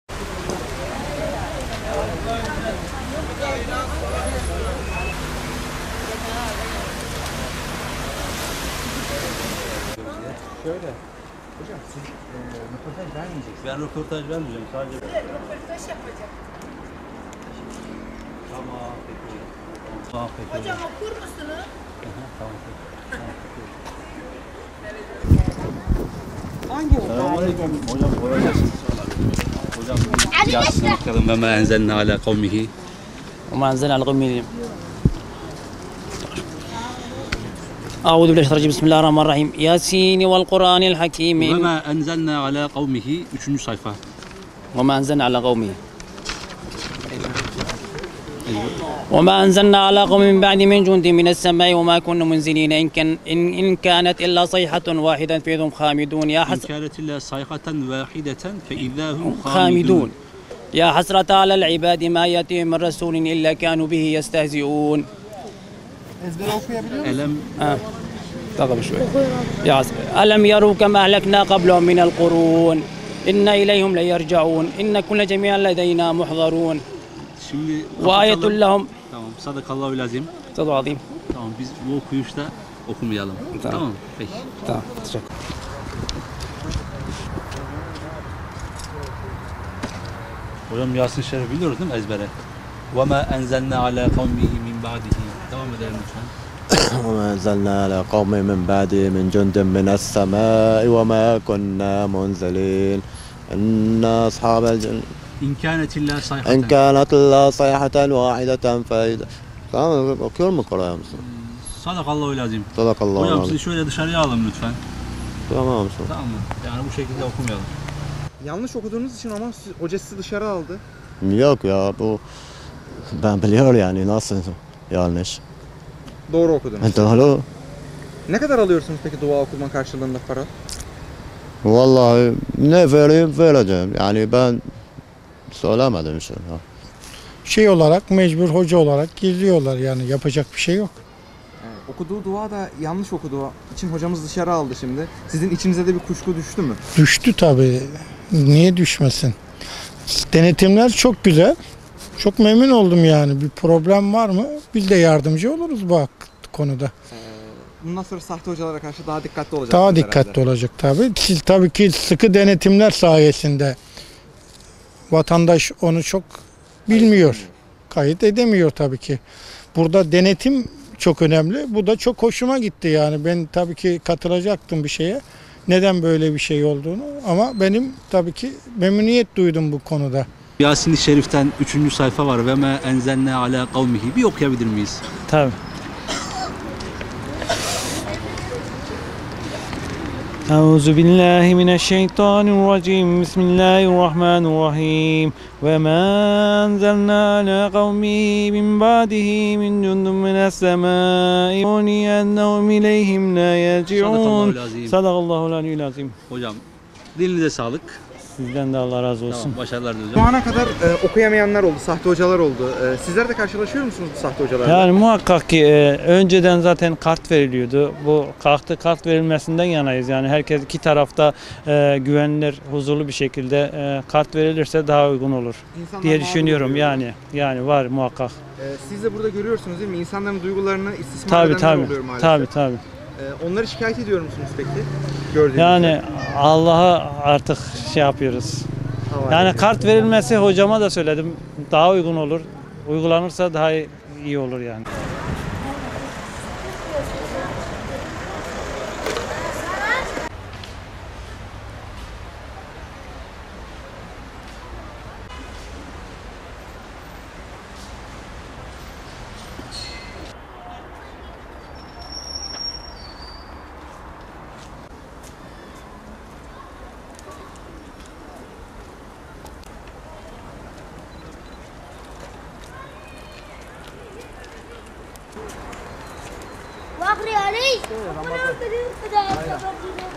Adana'da sahte hoca denetimi
Bayram ziyaretlerini fırsat bilip mezarlıklarda para karşılığı dua okuyan bazı kişilere denetleme yapıldı. Duaları okuyamayan sahte hocalar komik görüntüler oluşturdu.